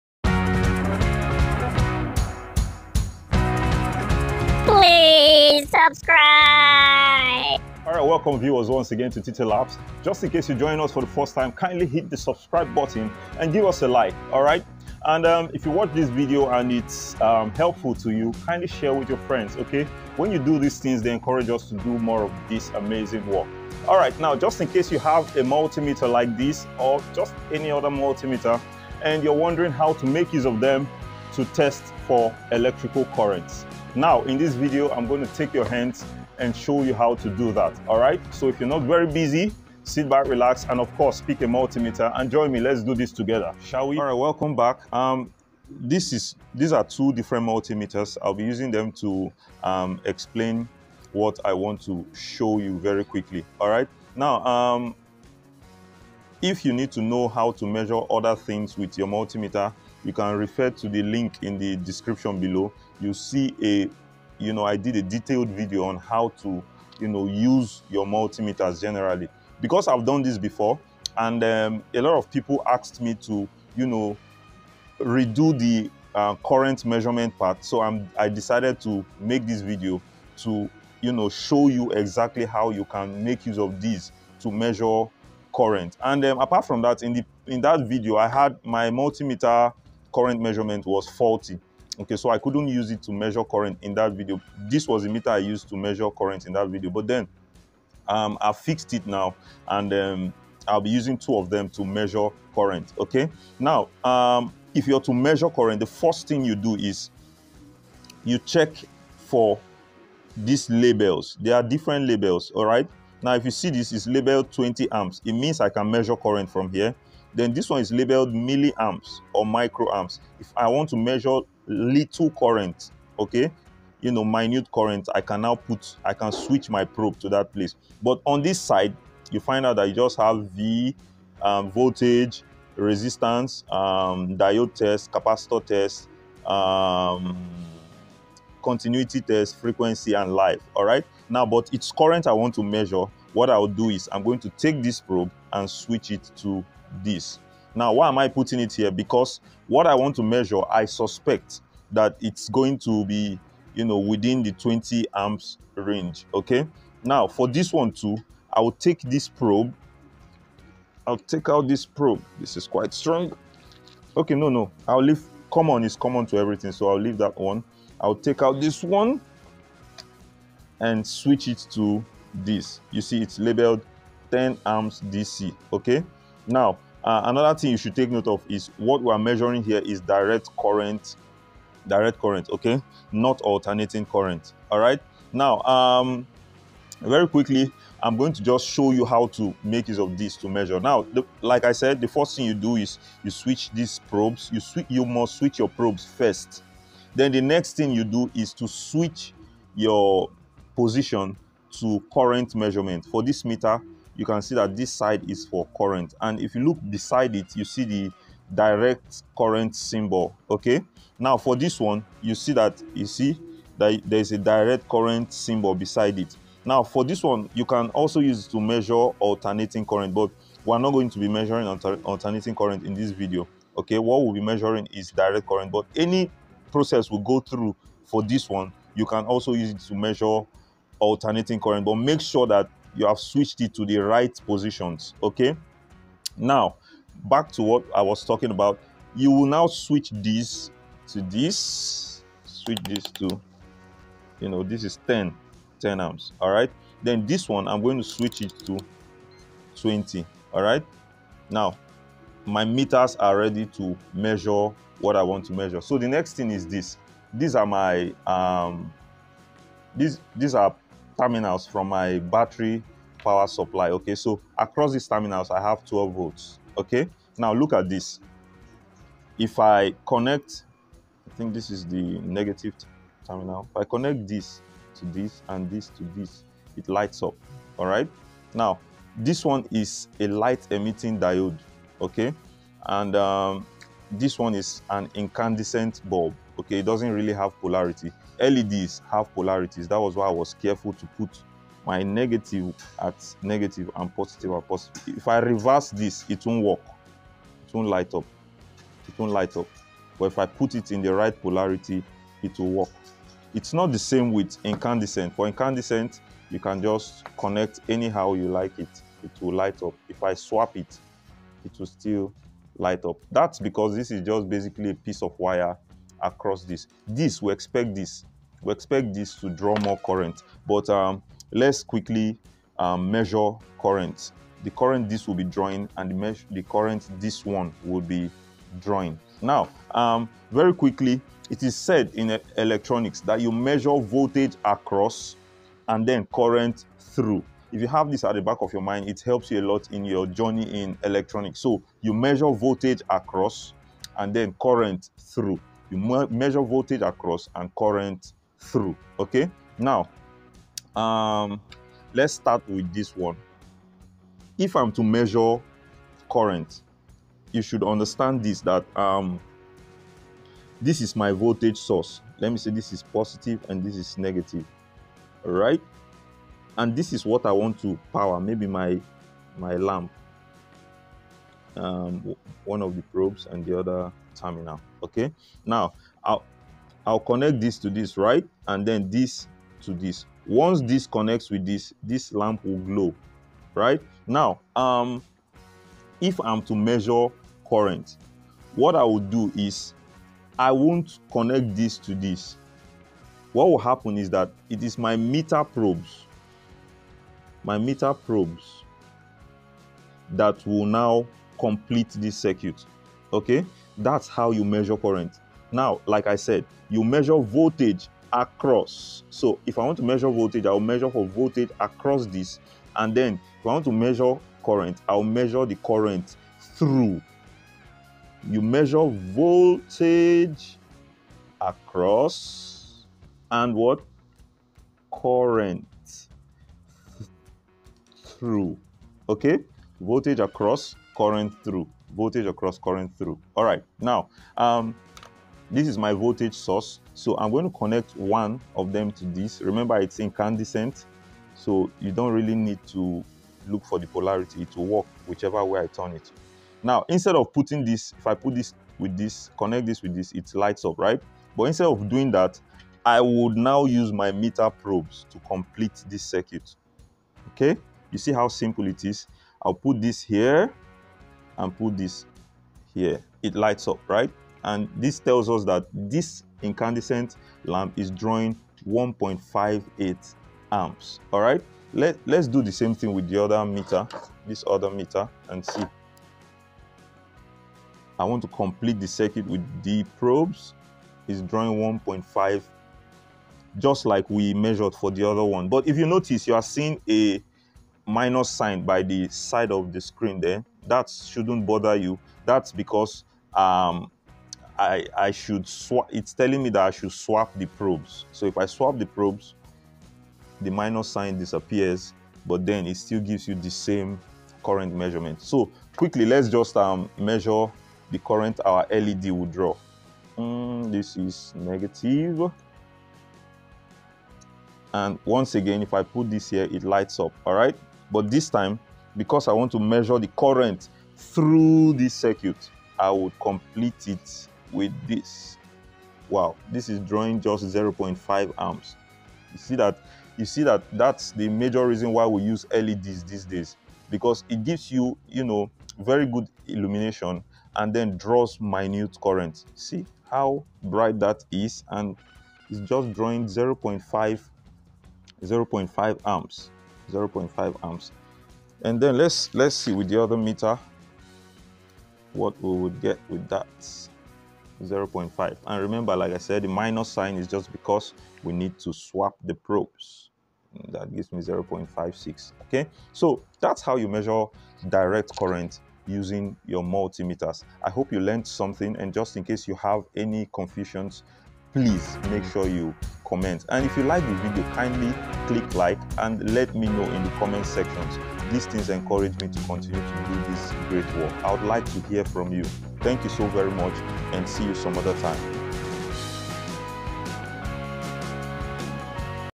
Measuring Current With A Digital Multimeter | How To Measure Amps Using Digital Multimeters
This video describes how to measure direct current using digital multimeters. It was made to further explain my previous video: " Digital Multimeter Tutorial"

Thanks and enjoy your amazing viewing experience. We're Teeta Tech, We Deliver!